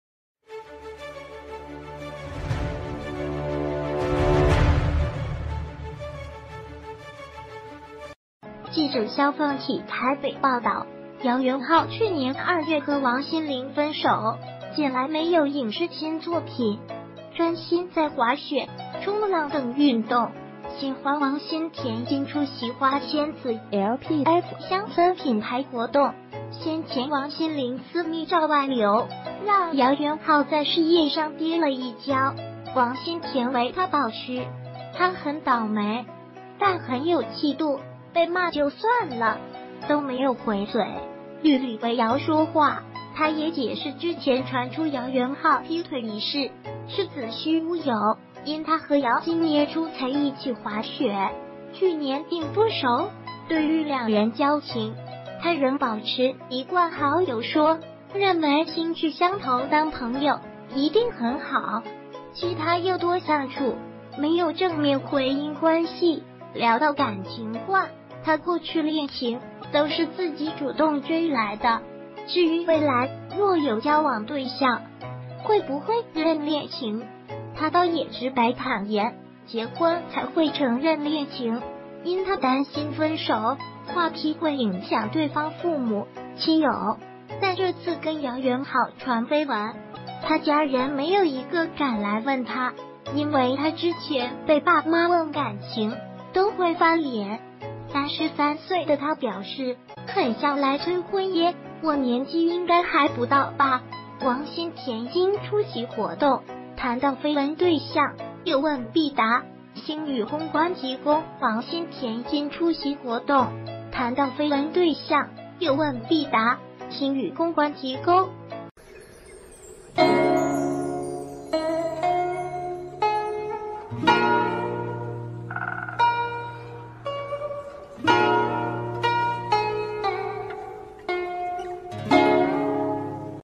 姚元浩私密照外流 王心恬為他抱屈「很有氣度」
〔記者蕭方綺／台北報...